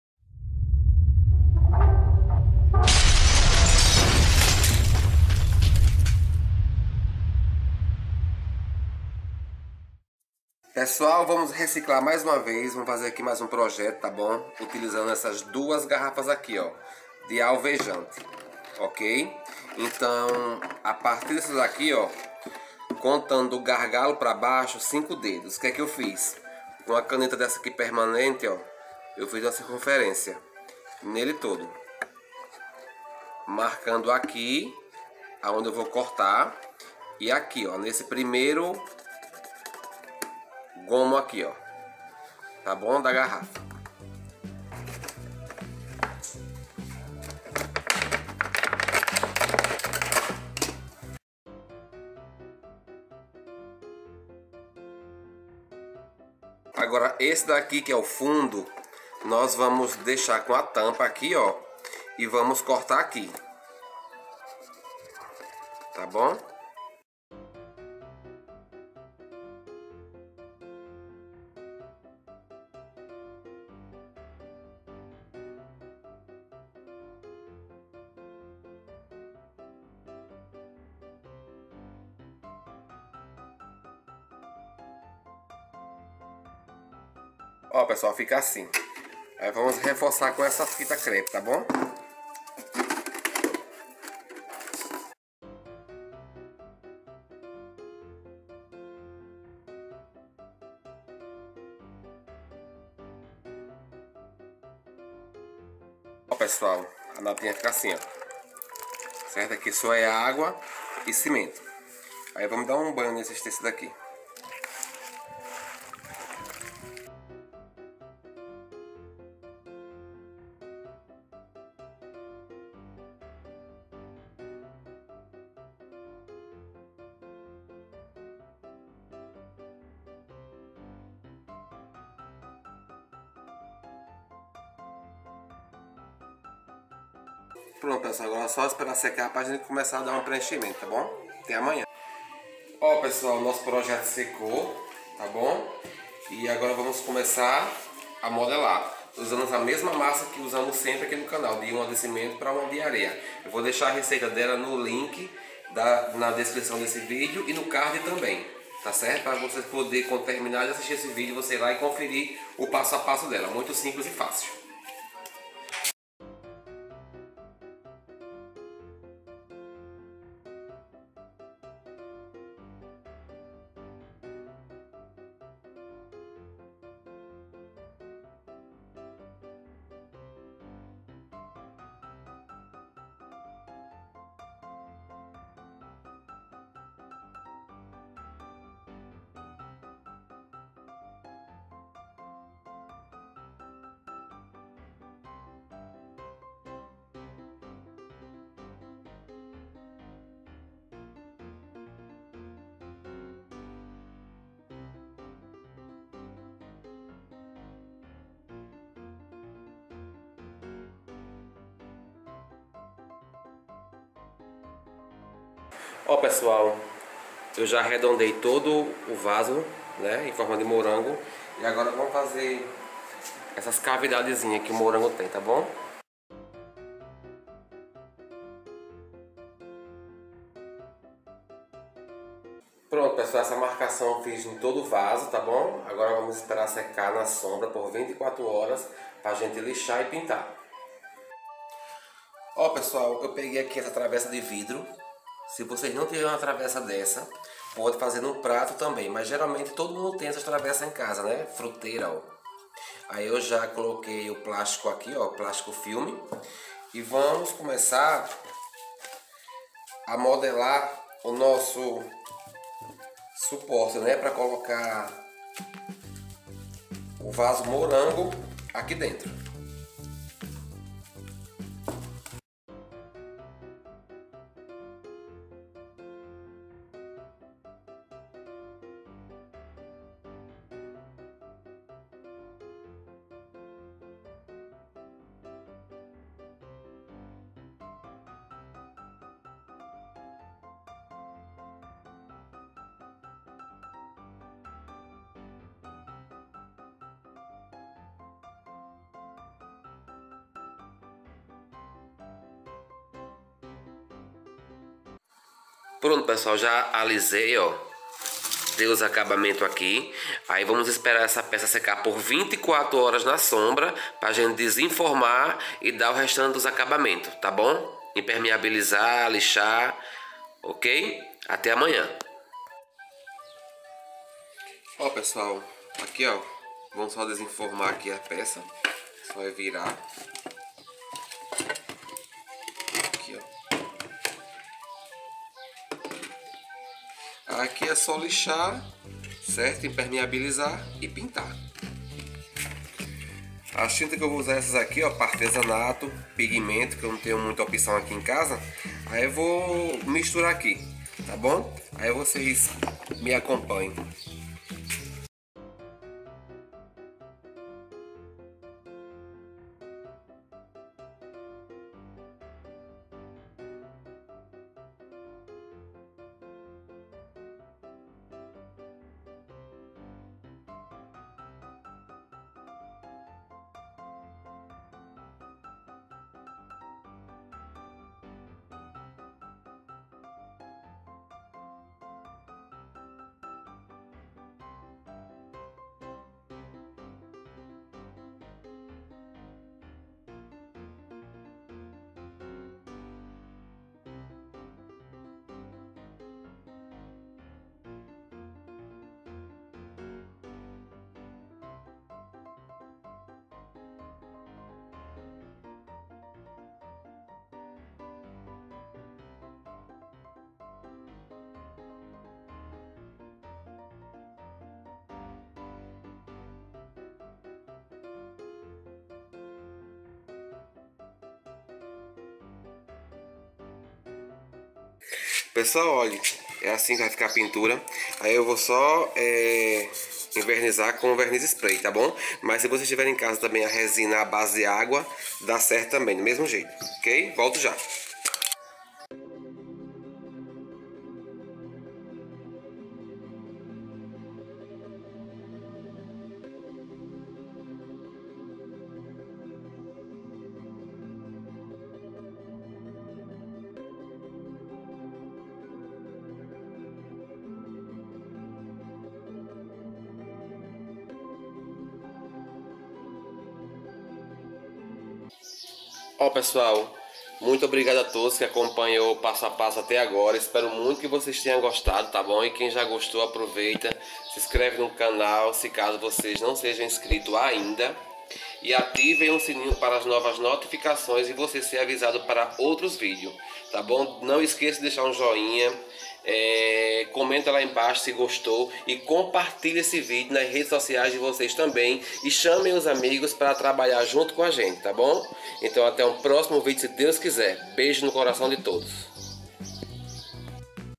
VASO MORANGO DE CIMENTO
Olá pessoal.
Fiz esse vaso e quero ver la no grupo do Facebook Nill Artes com Amor feito por vocês esse vaso em forma de morango e fiz também um suporte para o vaso feito com cimento também.
Os materiais usados: cimento, areia, pedaços de tecido, 2 garrafões vazios de desinfetante. uma travessa para fazer o suporte, tinta a gosto.
Receita da massa modeladora: 250 g de cimento, 250 g de areia ambos peneirados.